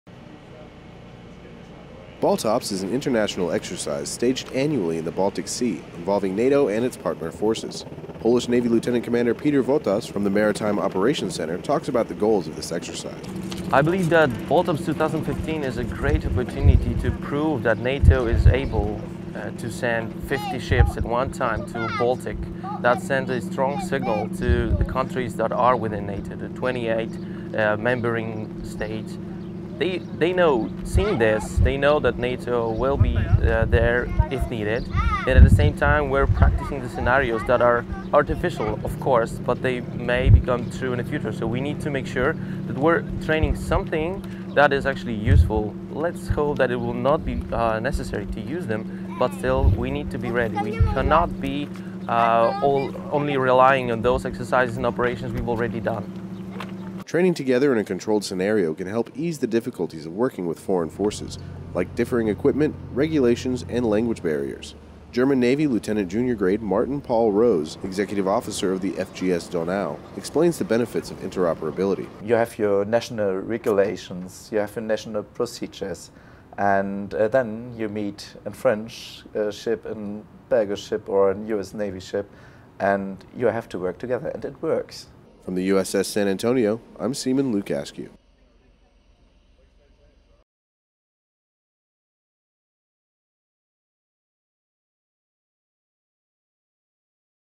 INTEROP
BALTOPS is an annual multinational exercise designed to enhance flexibility and interoperability, as well as demonstrate resolve among allied and partner forces to defend the Baltic region.